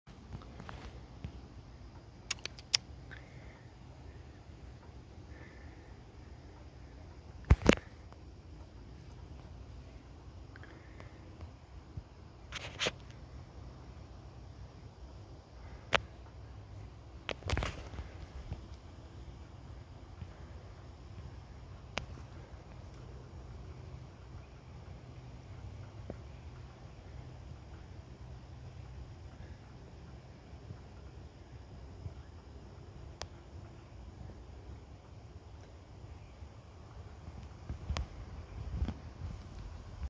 Beautiful Ontario White Tail Deer Eating | MTB Chronicles | September 9, 2021 (Close Up)
Spotted a geargous Ontario Female White Tail Deer whilst Mountain Biking in the evening. White Tail Deer Eating.